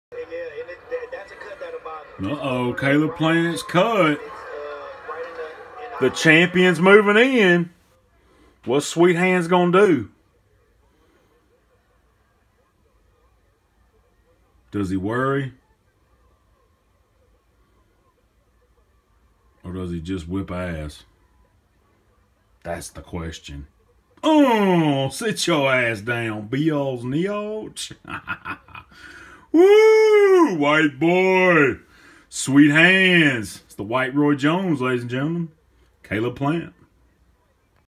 Caleb Plant knocks down Jose Uzcategui
2nd knockdown of the fight 1/13/19